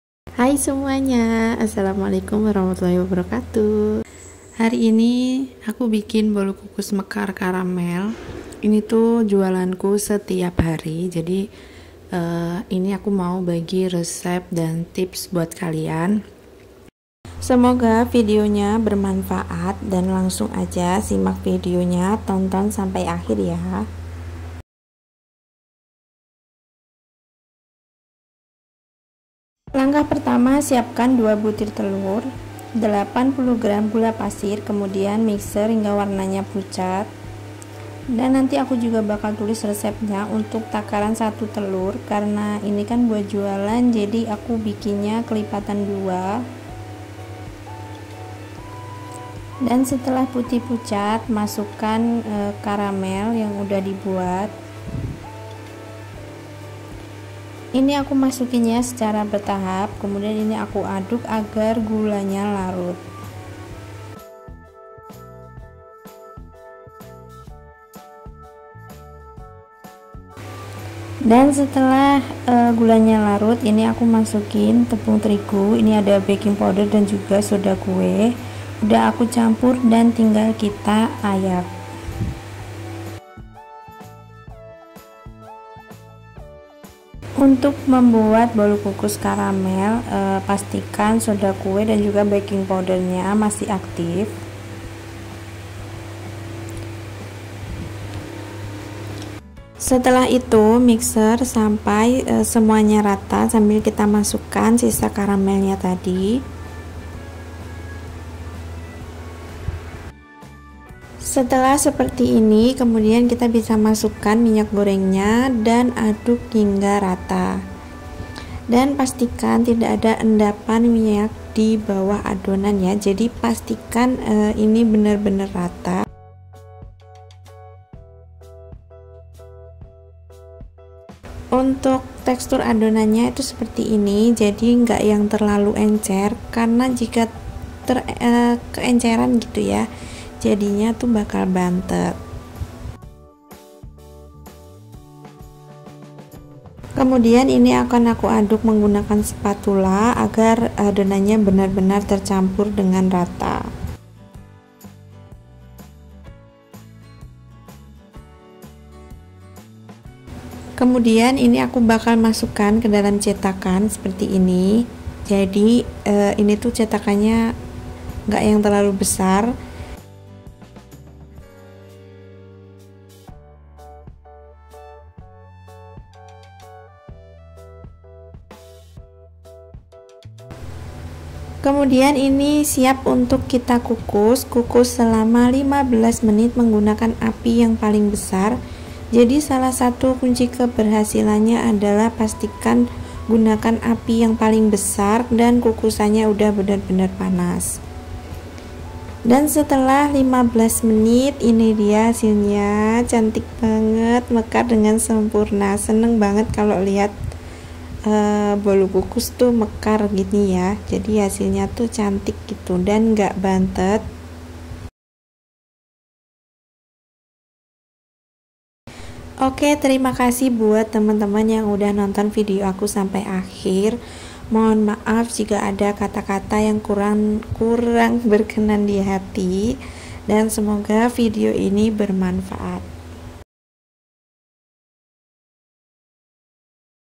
HANYA 1 TELUR BISA MEKAR || RESEP BOLU KUKUS MEKAR KARAMEL
Hay semuaaa
Tonton vidionya ya semoga bermanfaat

RESEP :
1. Bahan karamel

2. Adonan
 . 40 gr gula pasir
 . 1 btir kuning telur
 . 300 gr tpung terigu
 . Vanili sckpnya
 . Garam seckupnya

UNTUK 1 RESEP INI MENGHASILKAN 16/15 BOLU KUKUS MEKAR HARGA JUAL KU 2000 AN
JADI : 16 x 2000 = Rp 32.000